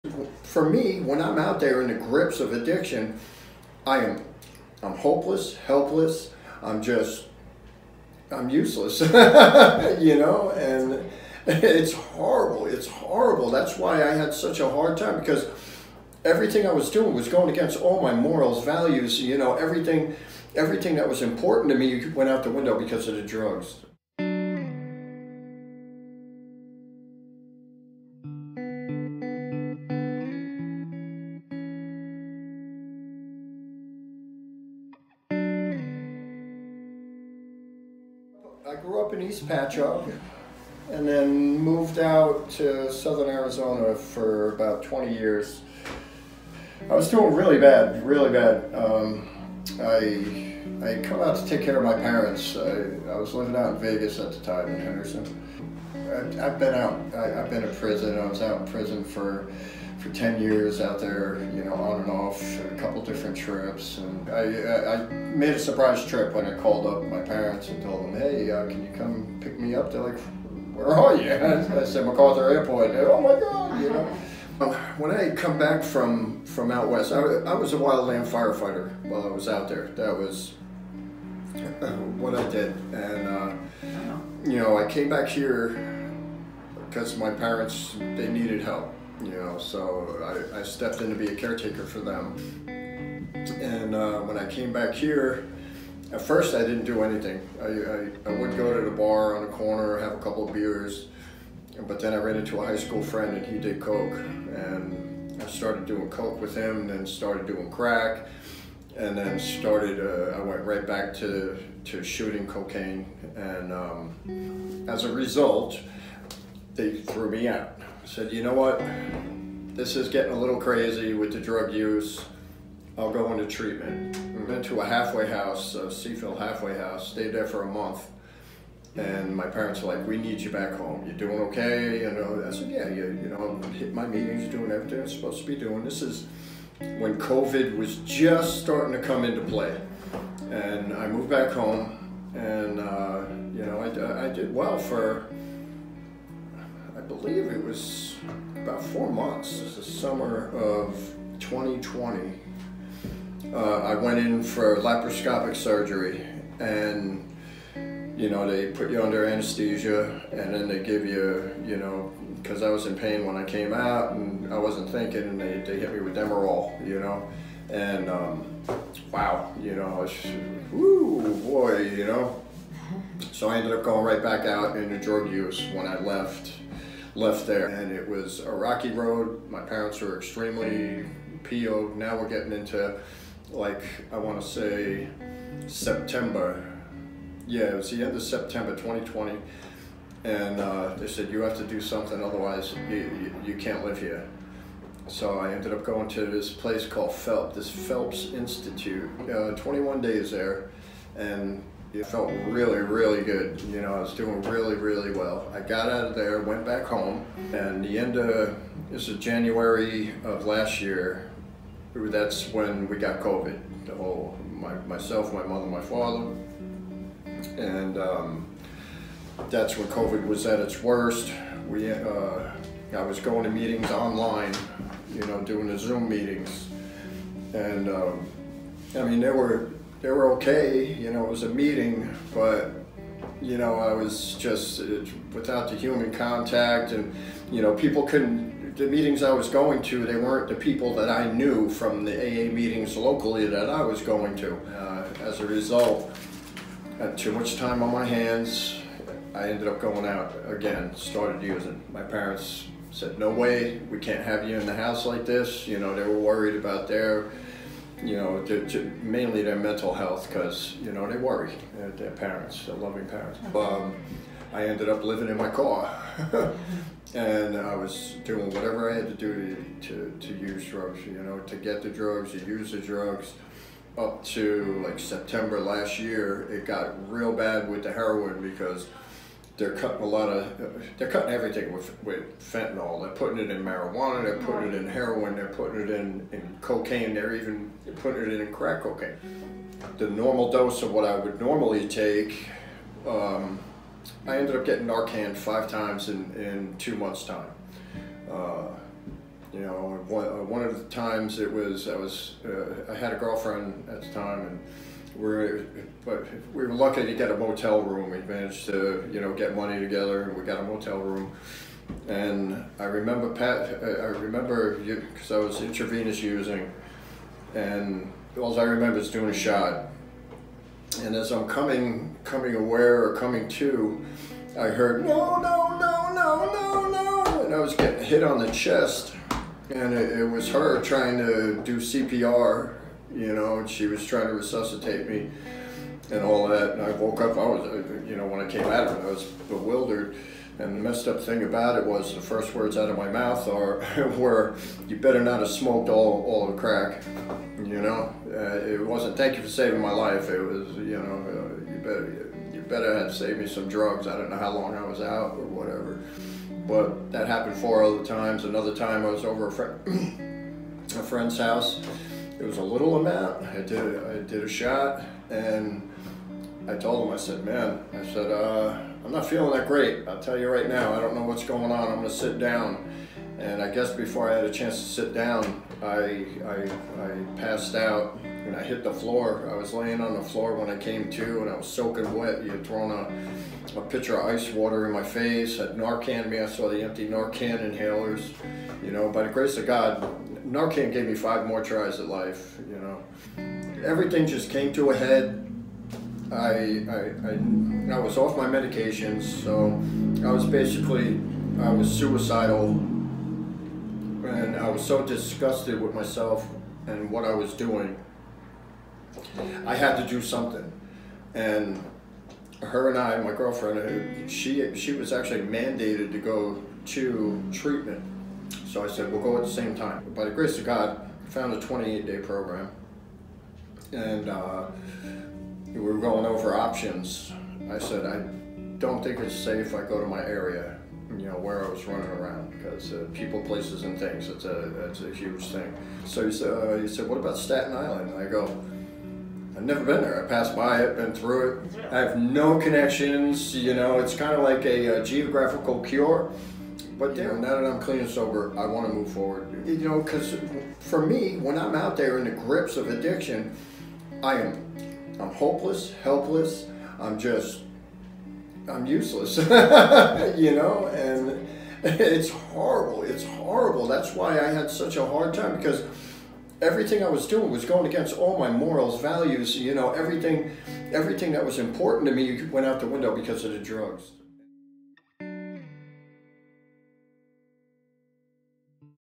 He thought he was using heroin, then he 'just dropped': Former addict recalls how he almost died
Christopher M. overdosed on fentanyl in 2021 when he injected what he thought was heroin into his body. (Video: Jan Somma-Hammel)